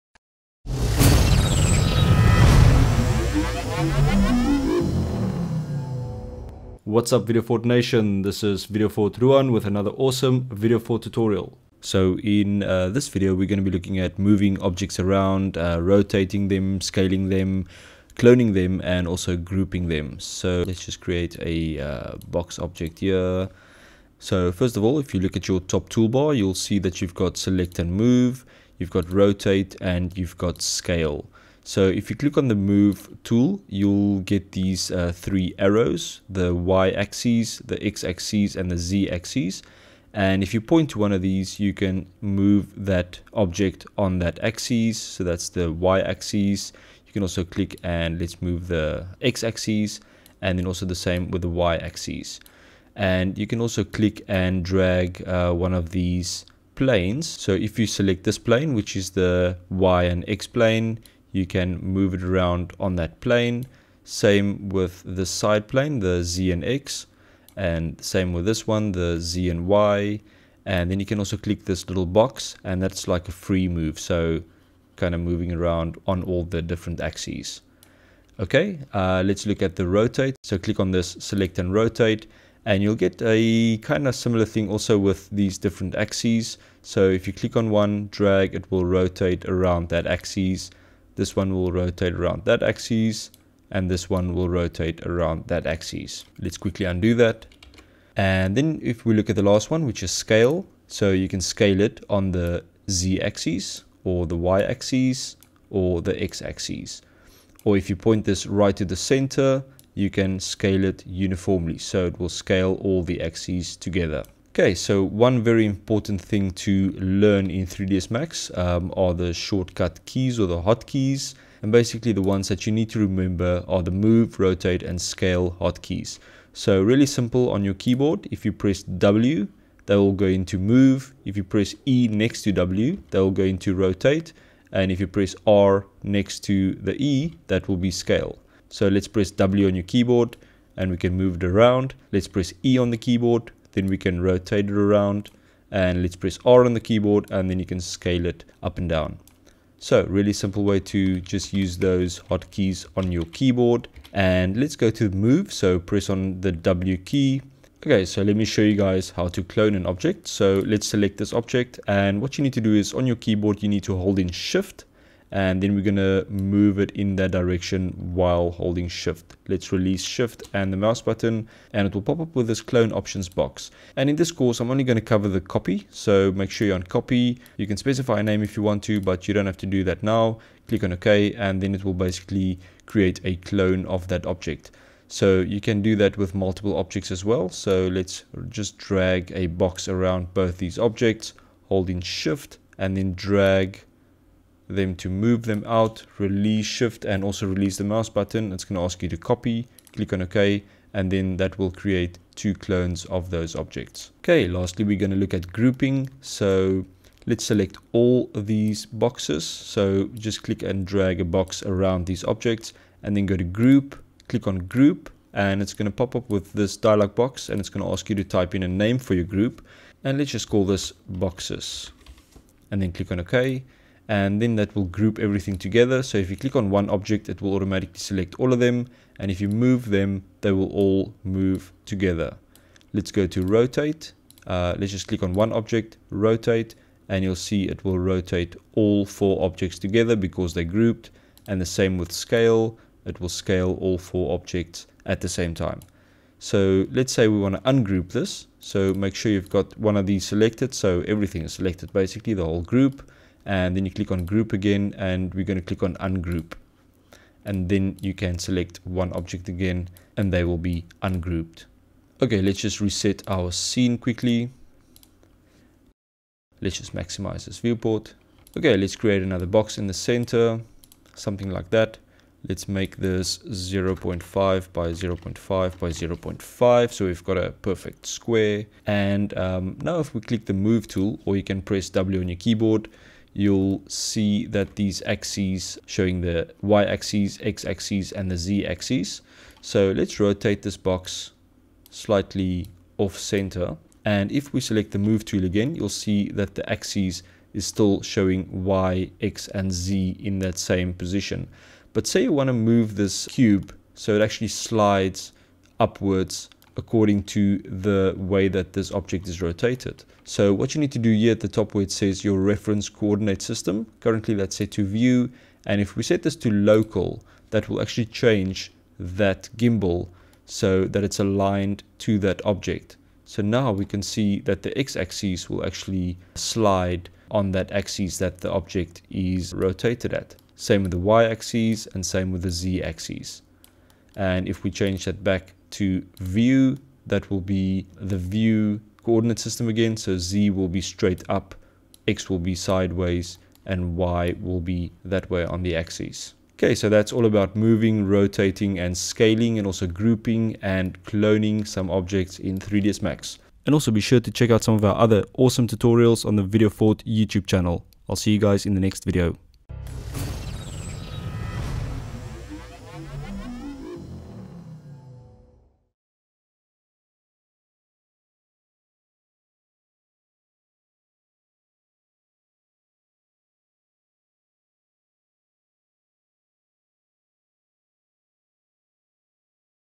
6 Move / Scale / Rotate / Clone
In this lesson you will learn how to generally manipulate an object in 3ds max using the move, scale, rotate, and clone tools. We will go over how to move an object along the X, Y, and Z axis, how to similarly rotate and scale an object along these same axes, and the hotkeys associated with each. Later in the tutorial we will cover how to clone objects, as well as how to group or ungroup multiple objects. Finally, you will learn how to use what you’ve learned on an object that is positioned off axis using the reference coordinate system drop menu. At the end of this lesson you should be ready to create your own 3d objects.